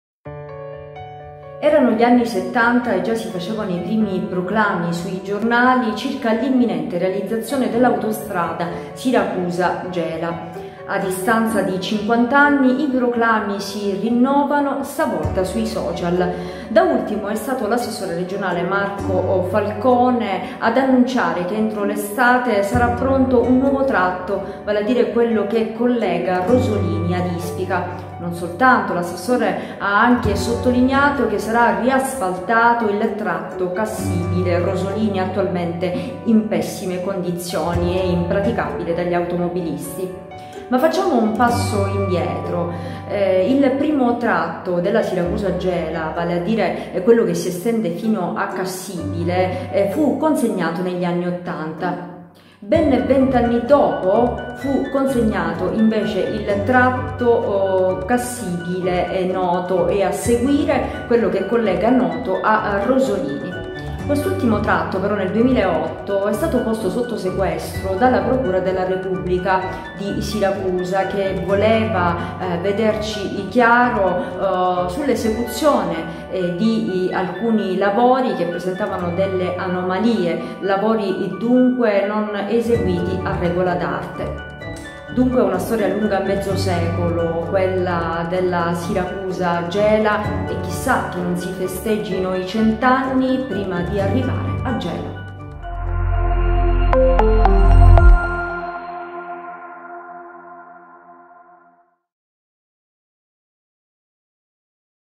Siracusa - Gela, odissea sull'asfalto. Un'autostrada lunga cinquant'anni
Nei proclami di fine anni '60 che si leggevano nei titoli a sei colonne dei giornali dell'epoca, l'autostrada Siracusa-Gela doveva essere completata nel 1973. A distanza di 50 anni, continuano i proclami (soprattutto su Facebook), ma l'autostrada è ancora lontana dall'essere terminata.
Nel filmato la ricostruzione veloce e puntuale di un'odissea targata Cenzina Salemi.